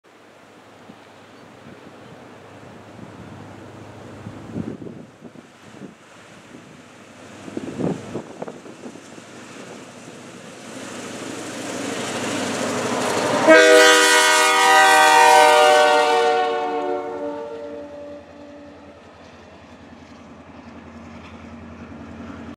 HD CMQ GP38-3 #3812 pulls the PAR OCS at Stateline 2/2/2018
The Pan Am Railways OCS crosses from MA to NH at Stateline with CMQ GP38-3 #3812 ST 101 & ST 100